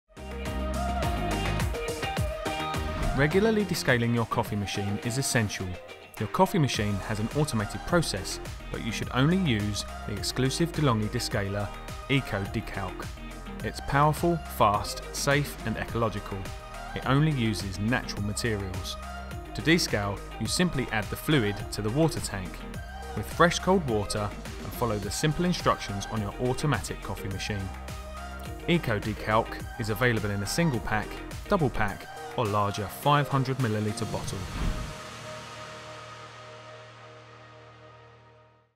How-To | EcoDecalk Descaler for Fully Automatic coffee machines
Regularly descaling your coffee machine is essential. Your coffee machine may have an automated process but you should only use the exclusive De’Longhi EcoDecalk descaler. It is powerful, fast, safe and ecological. De’Longhi EcoDecalk uses only natural materials.

To descale you simply add the fluid to the water tank, with fresh cold water and follow the simple instructions on your automatic coffee machine.